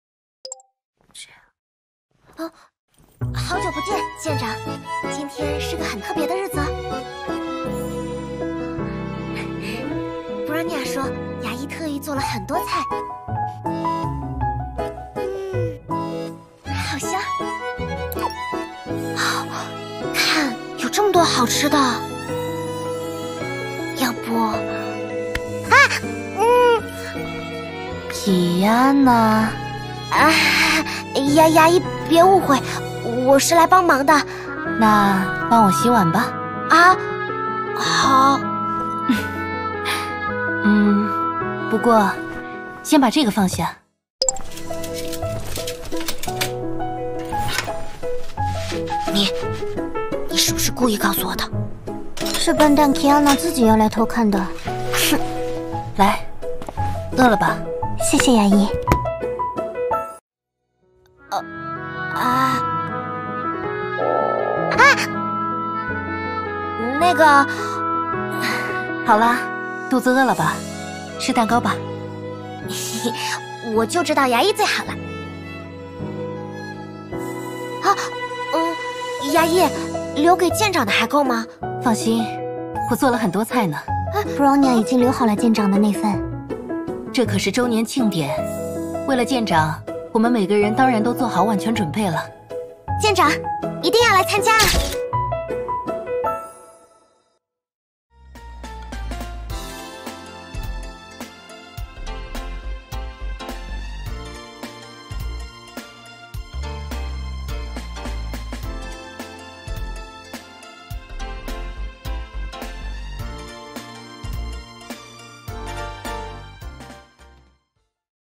Kiana's Vlog
This is full version of the 6.1 dev stream opening video.